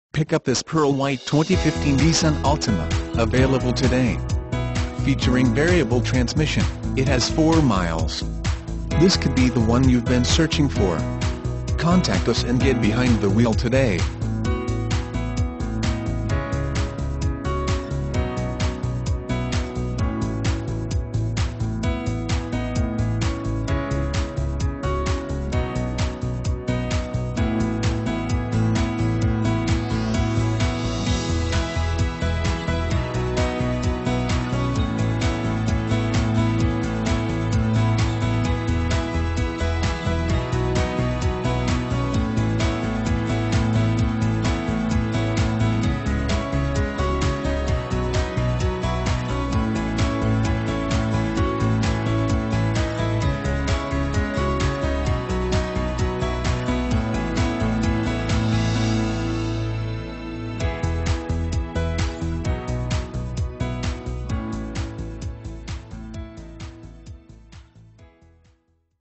2015 NISSAN ALTIMA Huntington, WV N15198
2015 NISSAN ALTIMA Huntington, WV
Stock# N15198

5200 US Rt. 60 E.

Pick up this Pearl White 2015 Nissan Altima, available today. Featuring Variable transmission, it has 30 miles. This could be the one you've been searching for.